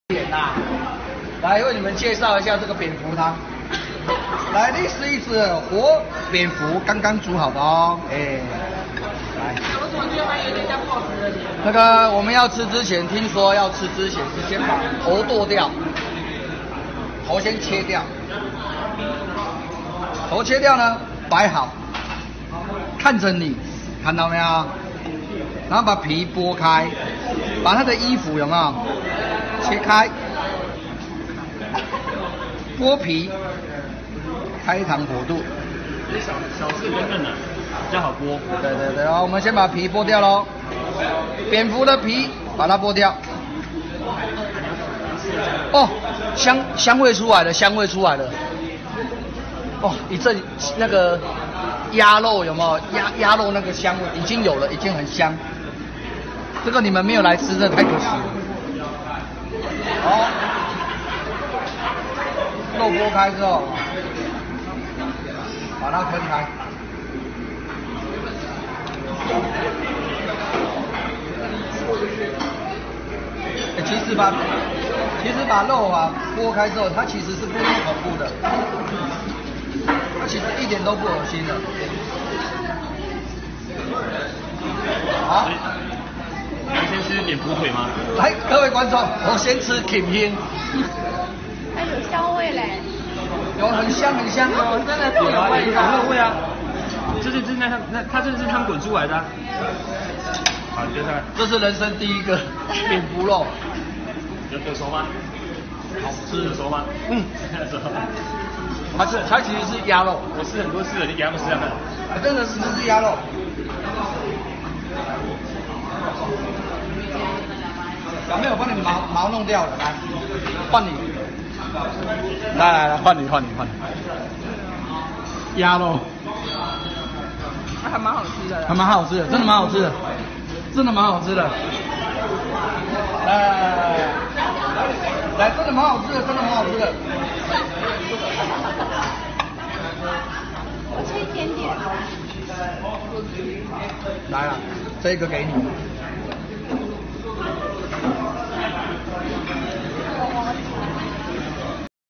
Chinese man eating cooked bat goes viral as the world combats Coronavirus
Man eating bat for the first time and shown how it's done.  His peers also agreed that this bat is a delicacy even though they did not like the impression of it.

The man shown how the bat is being prepared and the steps on how to eat it. It might be shocking at first , but that how people try exotic flavor of the wild to pay a hefty price to eat it.

Leave a comment on your view on this.